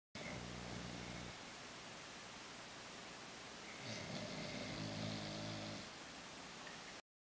Les ronfleuses
Bulldogs anglais,mère et fille ronflent en coeur